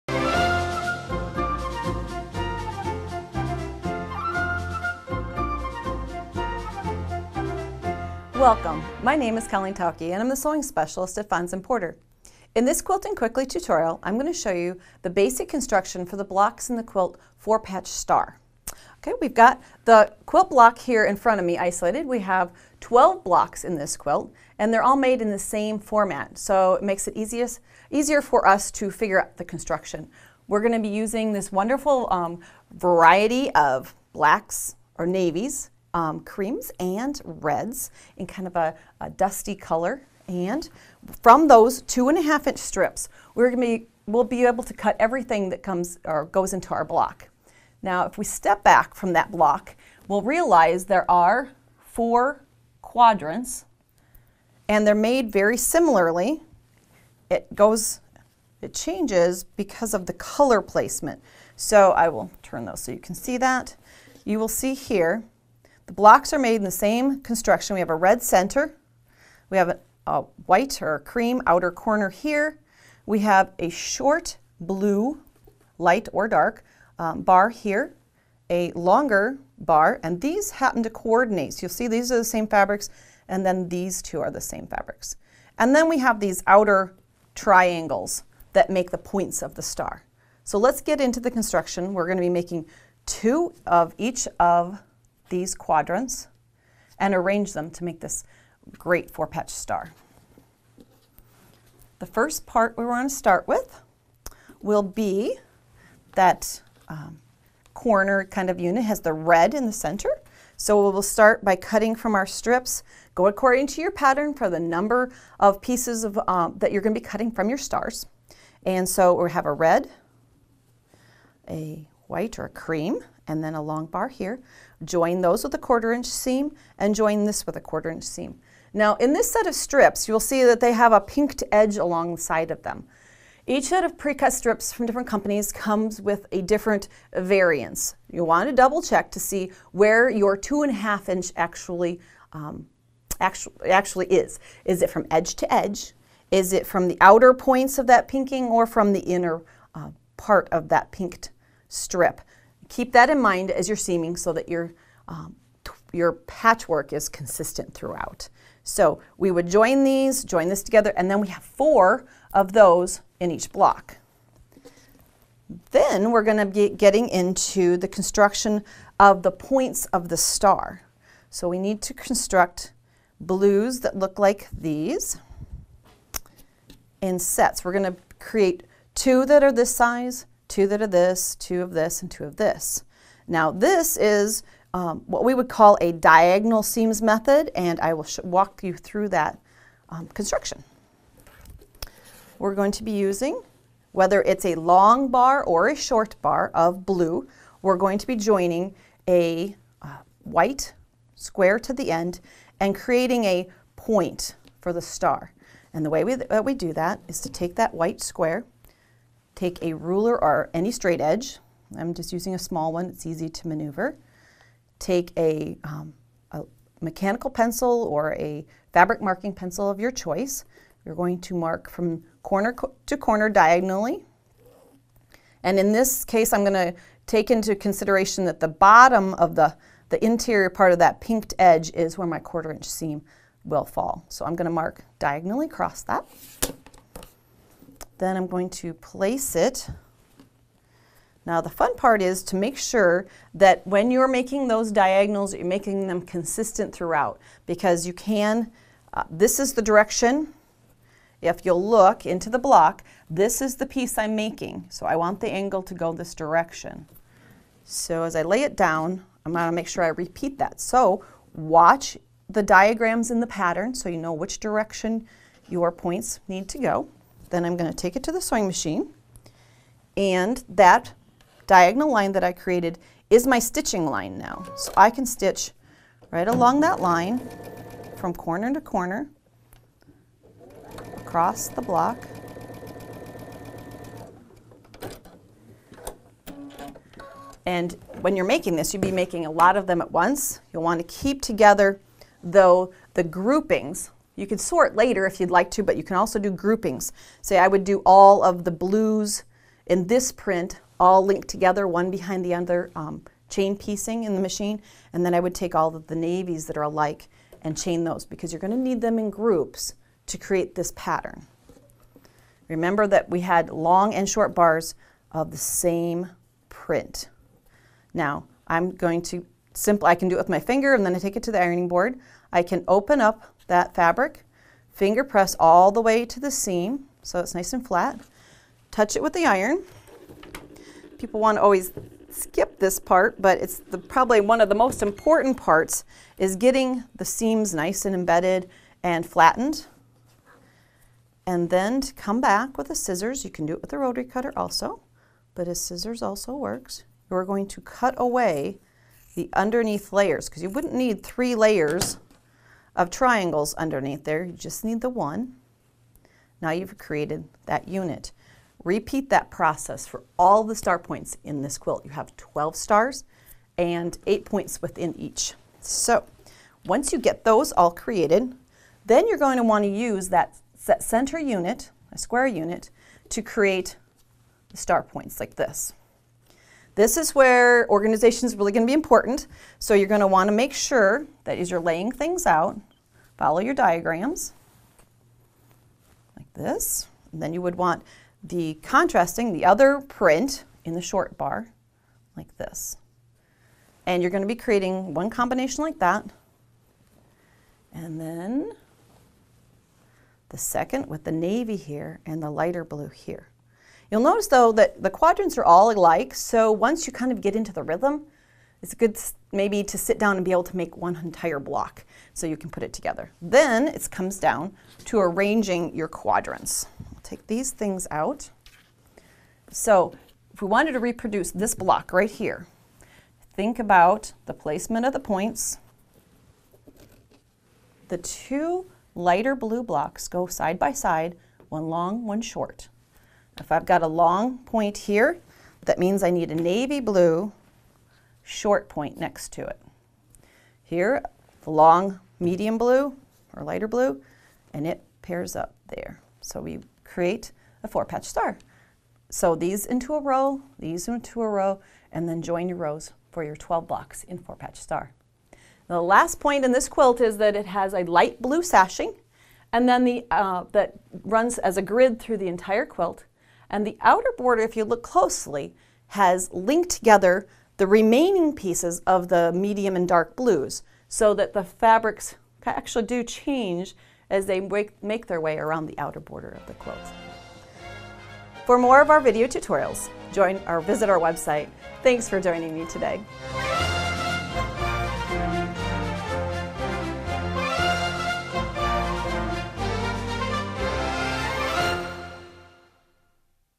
Quilting Quickly: Four-Patch Star Quilt - Star Patterns
Four Patch Star digital pattern Quilting Quickly November/December 2015 Quilts of Valor digital issue
Four-Patch Star is a fun star quilt. Star patterns come in all sorts of arrangements, but this one is exceptional. Use a collection 2½“ strips (commonly called a jelly roll or a roll-up) or make it in your favorite fabric collection from your stash!